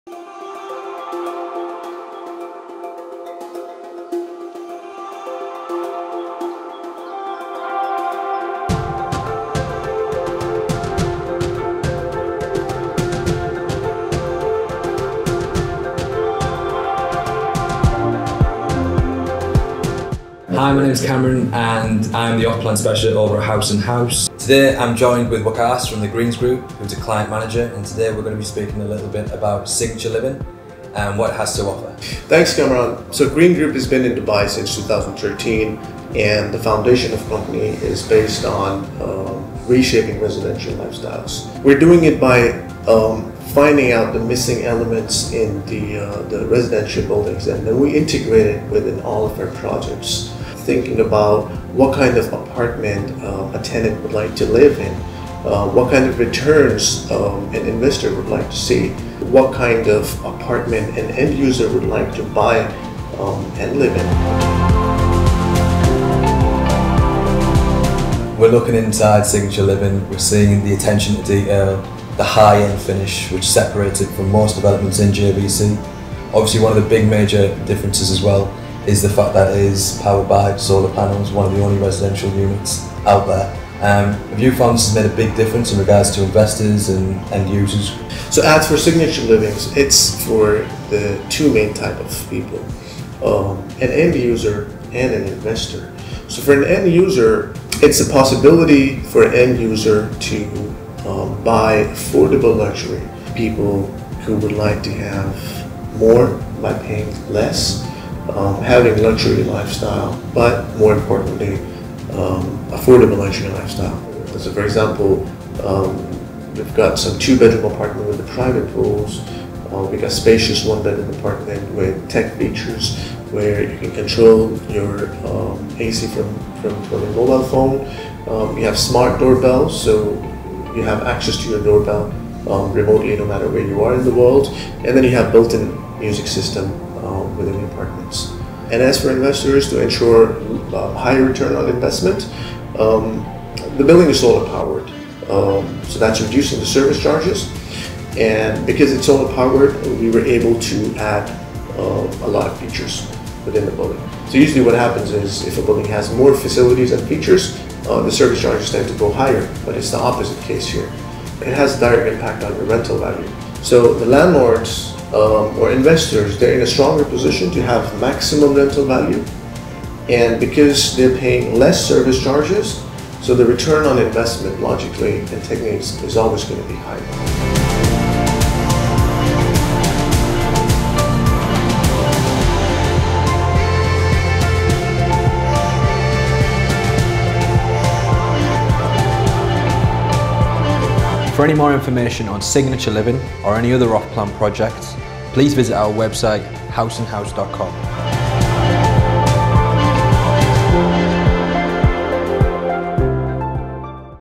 Signature Livings in JVC
There is limited supply left of these fantastic, eco-friendly homes in the heart of JVC. With top notch finishing, smart home features, attractive payment plans and a 2020 completion date – this is one project you will not want to miss. Our off plan specialist Cameron talks you through the key benefits of this fantastic investment in this short video...